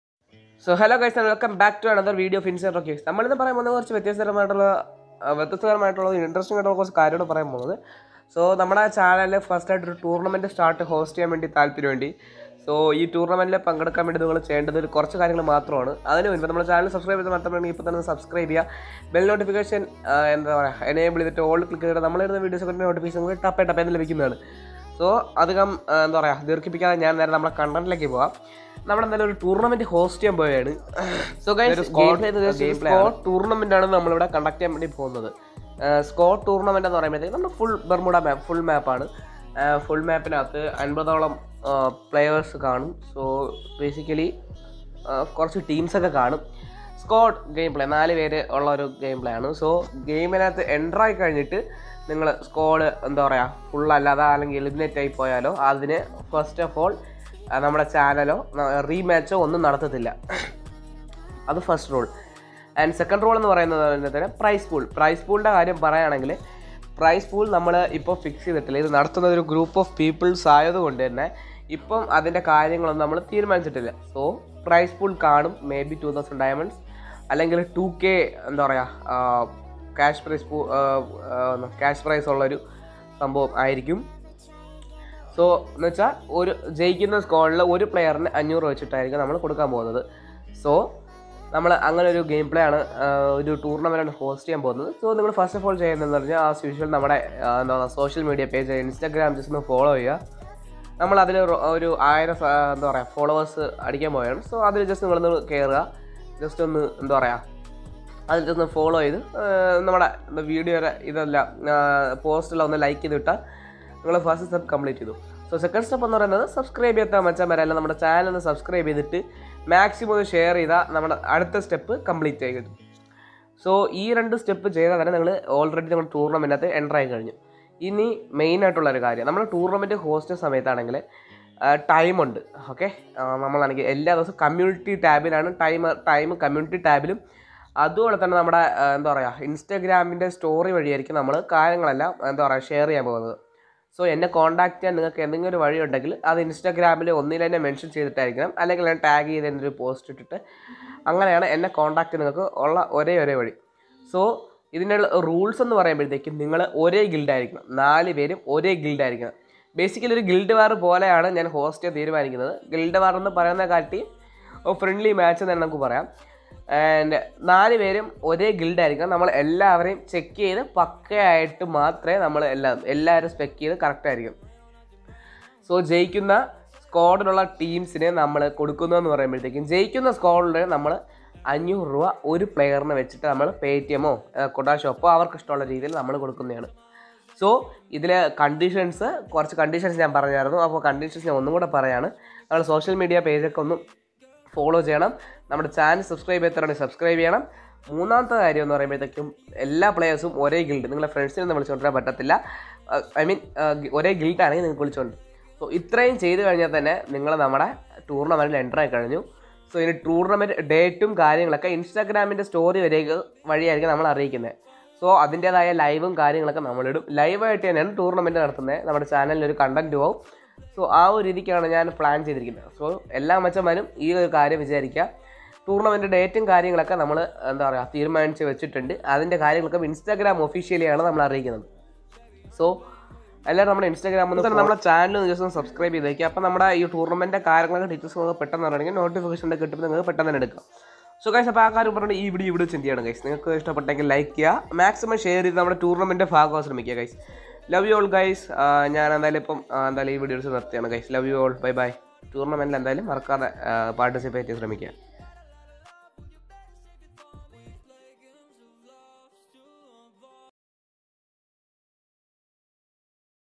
GIVE AWAY + GUILD TOURNAMENT DETAILS|| INSANE ROCKY
GOOYS WE ARE HOSTING A TOURNAMENT. FULL MAP CLASSIC SQUAD MATCH. ALL DETAILS WILL BE INFORMED ONLY ON INSTAGRAM.

 RULES:

🔰 SQUAD PLAYERS MUST BE IN ONE GUILD.

 PRIZE POOL : 500 PER PLAYER OF WINNING SQUAD. 🎯